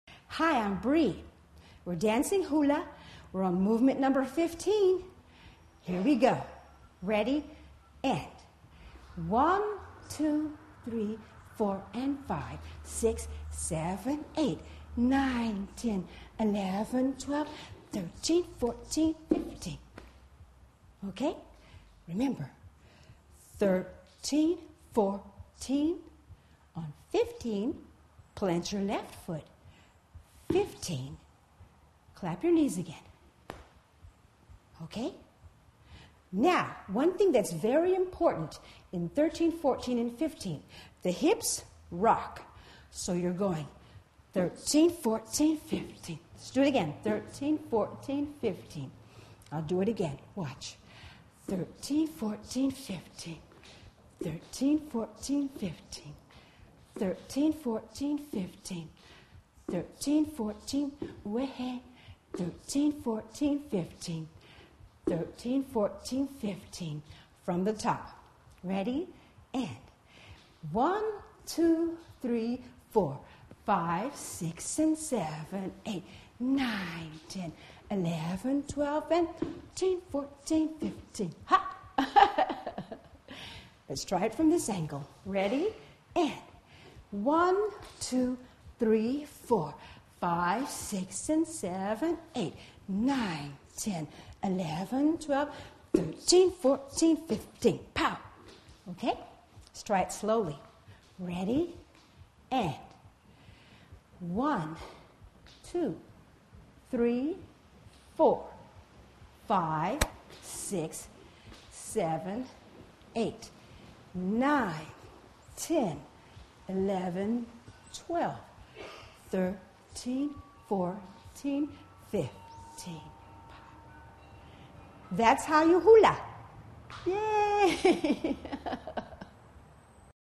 Tahitian Hula Dancing: Uwehe Movement
Tahitian Hula Dancing: Uwehe Movement. Part of the series: Tahitian Hula Dancing. The Uwehe Movement in hula dancing requires rocking the hips from side to side while clapping the knees together. Learn the Uwehe with tips from a hula performer in this free dancing video.